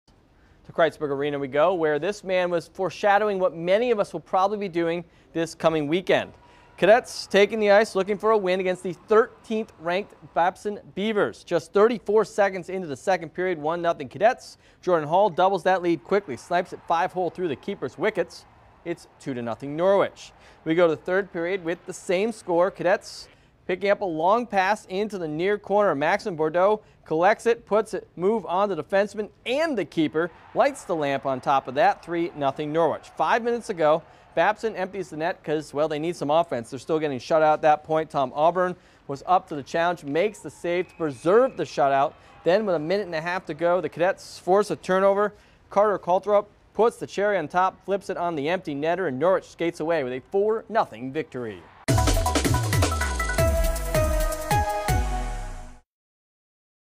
Cadets put the clamps on No. 13 Babson
Norwich skates to a 4-0 shutout victory over the 13th-ranked Beavers at Kreitzberg Arena.